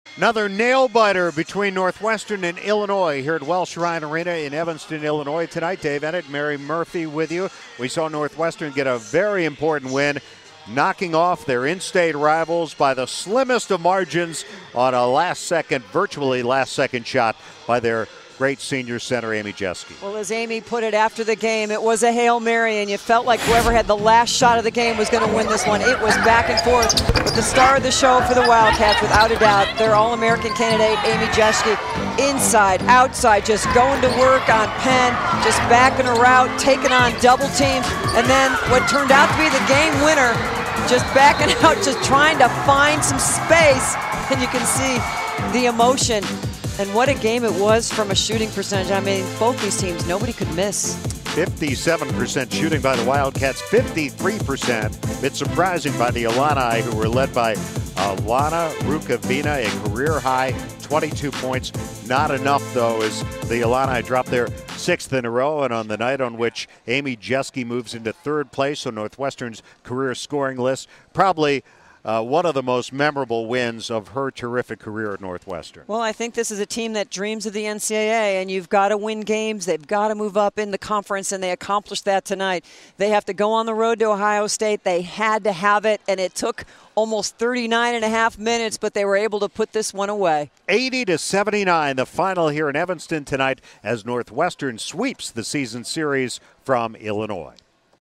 Illinois at Northwestern | Highlights | Big Ten Women's Basketball | Feb. 3, 2011
Northwestern beat Illinois 80-79 to sweep the season series from the Illini.